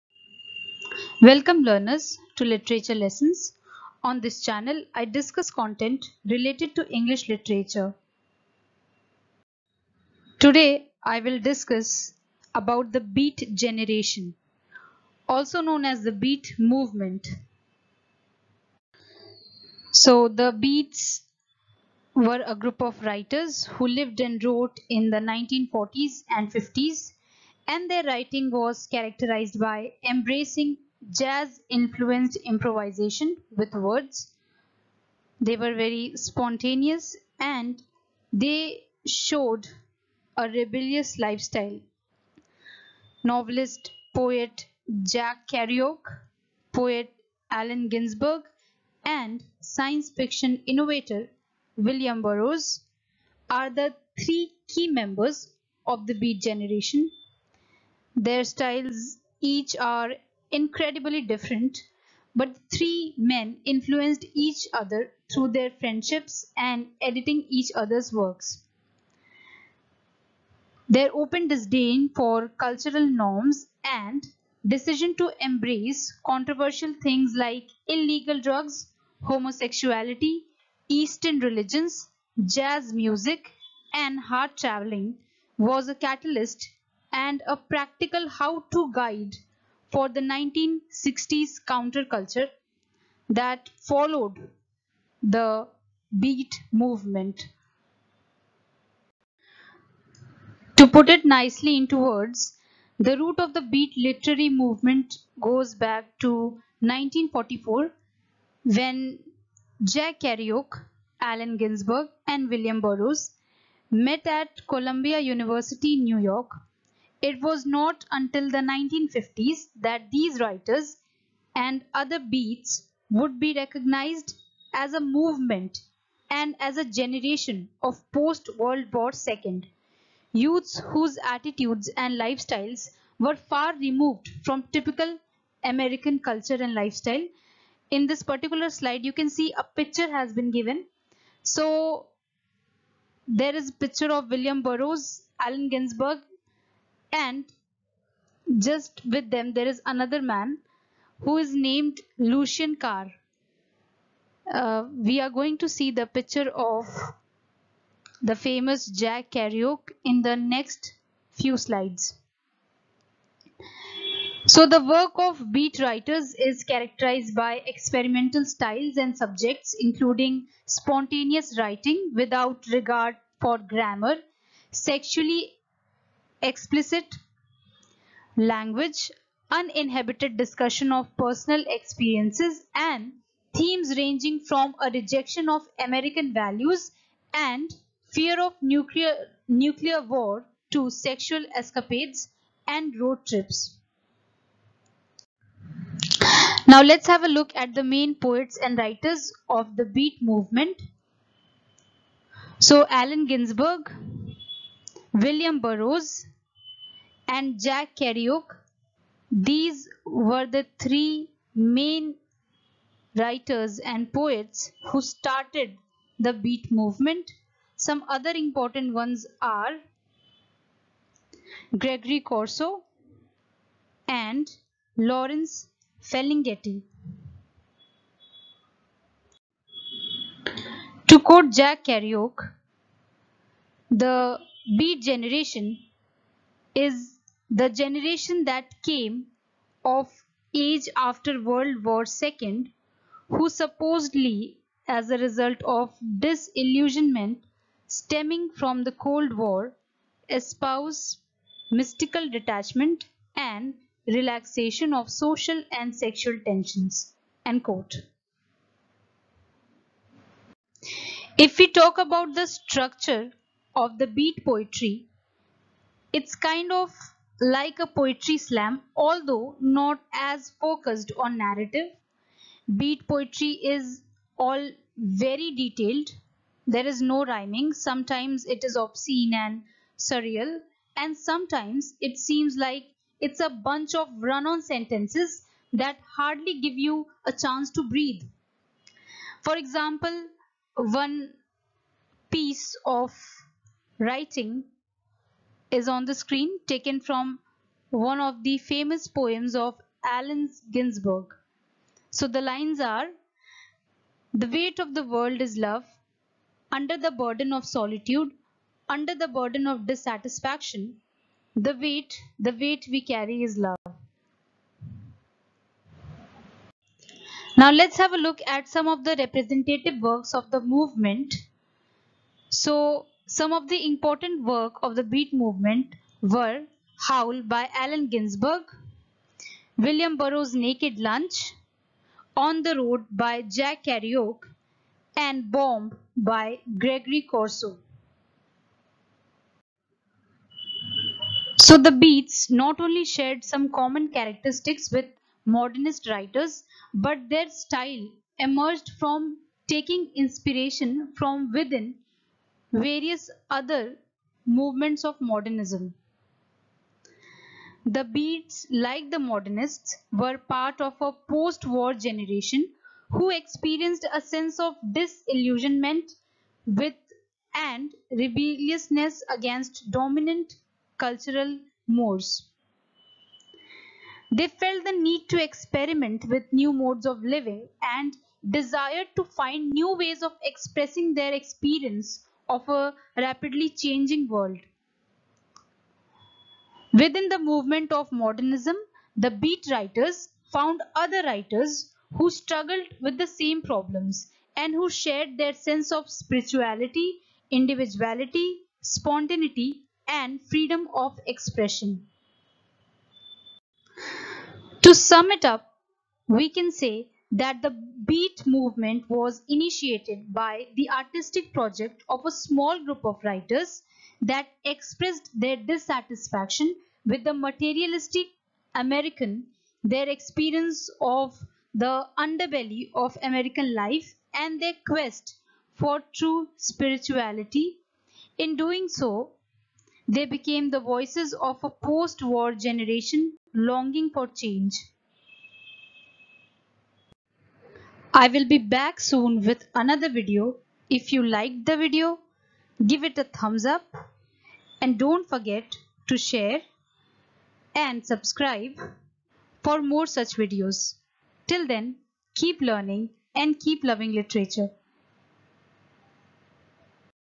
THE BEAT GENERATION/THE BEAT MOVEMENT
Hello Learners. This video is an introductory lecture to the Beat generation/ Beat movement. The origin of the movement, its major characteristics, some important writers and poets, and their works have also been discussed in the video.
 Happy learning.
Fun Fact : The movie 'Kill your Darlings' is based on the real life events of the Beat poets and writers . It centers around the famous ,Lucian Carr murder case. The movie is worth watching and gives a deep insight into the lives of the Beat Generation writers.